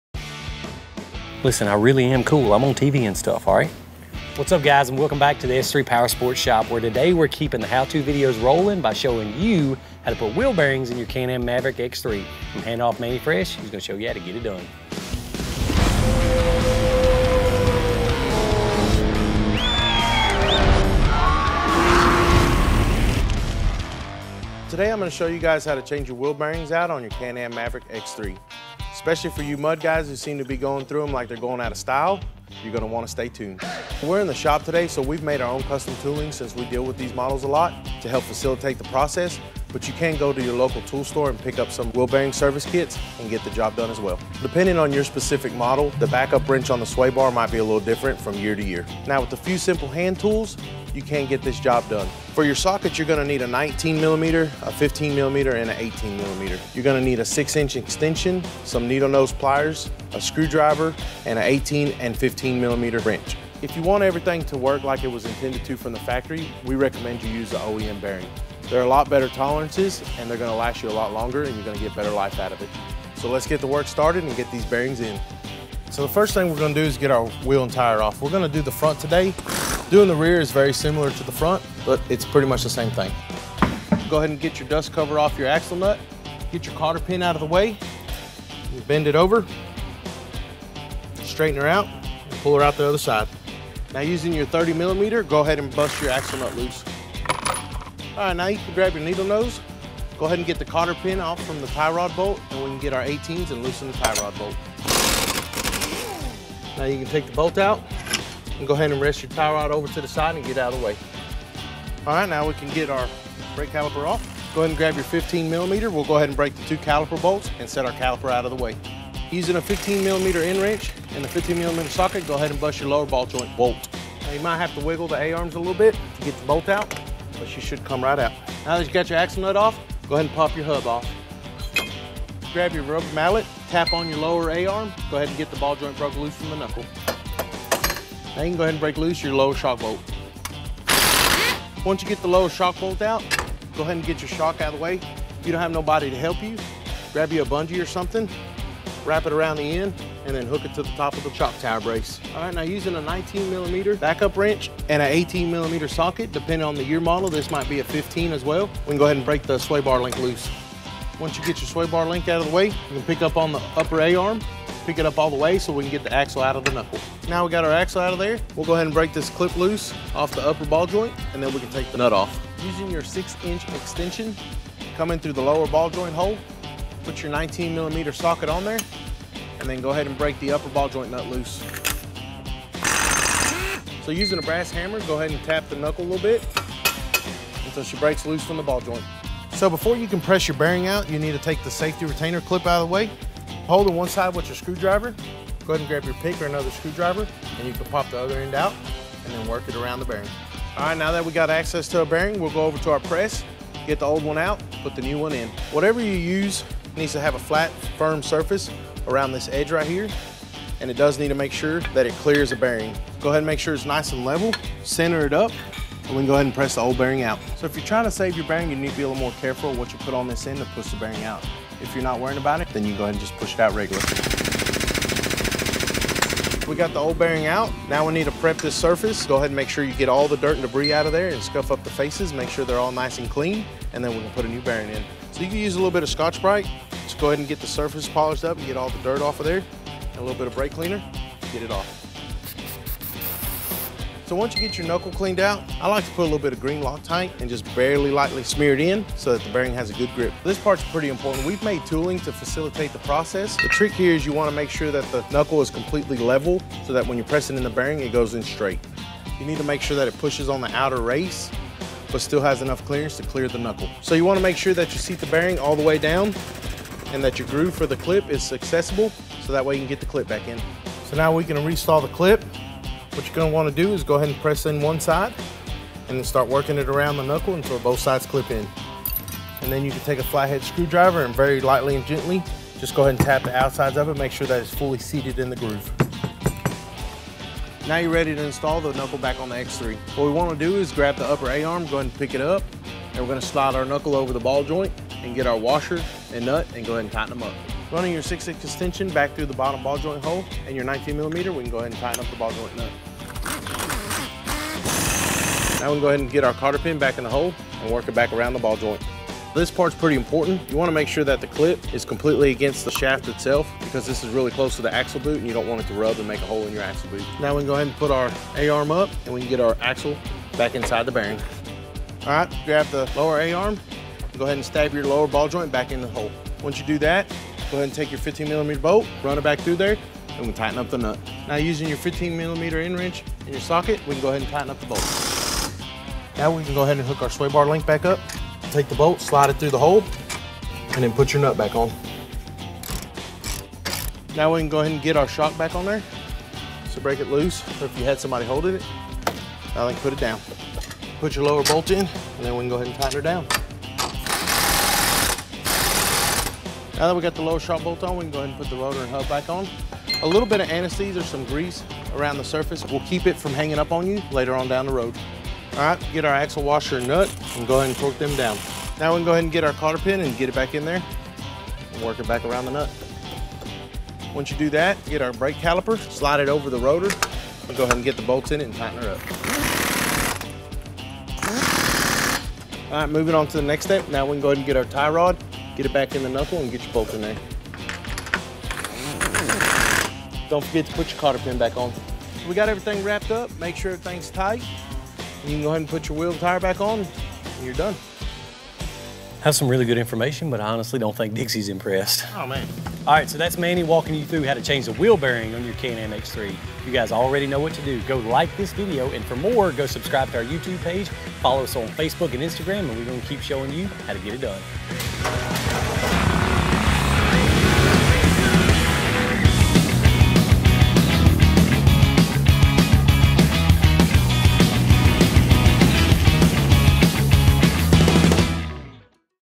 How to change the Wheel Bearing on your Can-Am Maverick X3
Feb 20, 2020  Have you ever had too much rock n’ roll in your wheel? We’ll show you the easiest method to change the wheel bearing on your Can-Am Maverick X3! Join Dustin and Manny at the S3 Power Sports shop and learn all our tips and tricks to help you get it done!